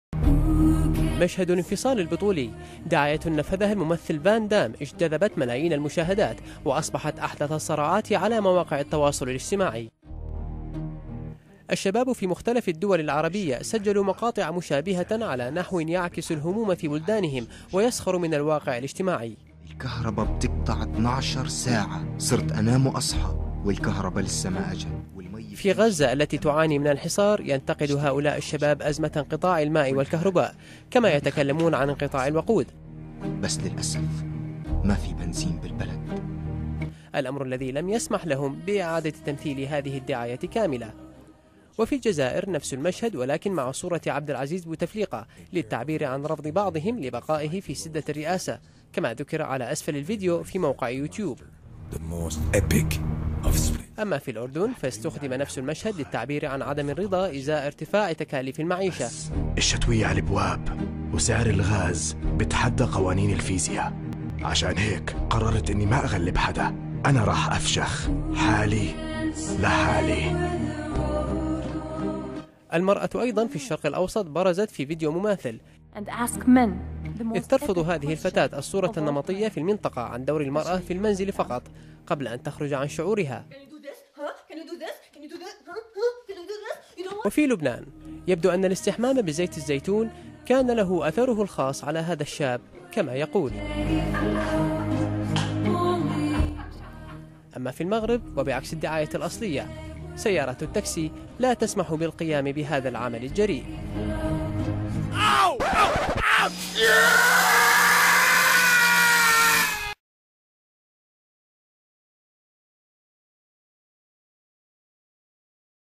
شباب عرب يقلدون إعلانا لفان دام للتعبير عن همومهم
قلد شباب في الشرق الأوسط حركة الممثل فان دام في إعلانه الجديد لشاحنات، للتعبير عن همومهم السياسية والاقتصادية